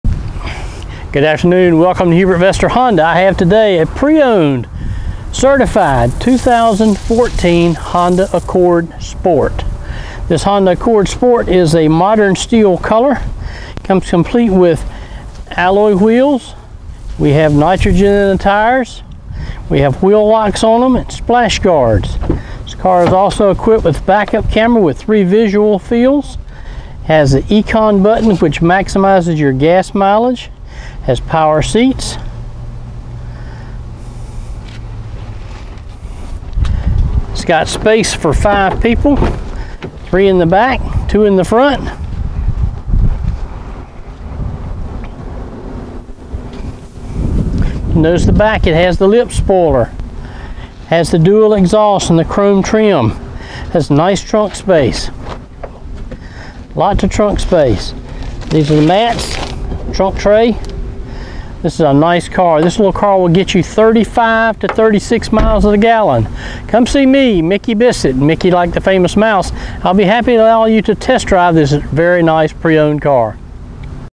2014 Honda Accord Wilson NC Walkaround
You won't find a nicer 2014 Honda Accord than this one-owner gem. This fantastic Honda is one of the most sought after used vehicles on the market because it NEVER lets owners down. Green Car Reports the 2014 Accord a 'Best Car to Buy.'
Clean CarFax, one owner with alloy wheels, new tires, rear backup camera, power seat, power windows and locks, and cruise control. Excellent fuel economy and budget friendly.